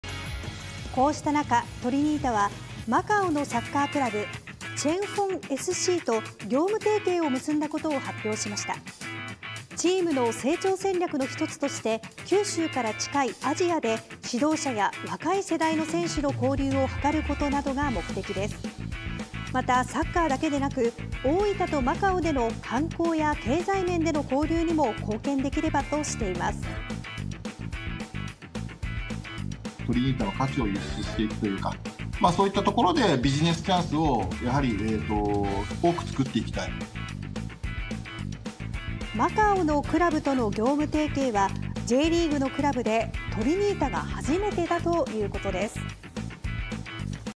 トリニータ マカオのサッカークラブと業務提携 Jリーグのクラブで初 大分
トリニータはマカオのサッカークラブ「青鋒SC」と業務提携を結んだことを発表しました。

チームの成長戦略の一つとして、九州から近いアジアで指導者や若い世代の選手の交流を図ることなどが目的です。

また、サッカーだけでなく、大分とマカオでの観光や経済面での交流にも貢献できればとしています。

◆大分トリニータ　小沢正風代表
「トリニータの価値を輸出しビジネスチャンスを多く作っていきたい」

マカオのクラブとの業務提携はJリーグのクラブでトリニータが初めてだということです。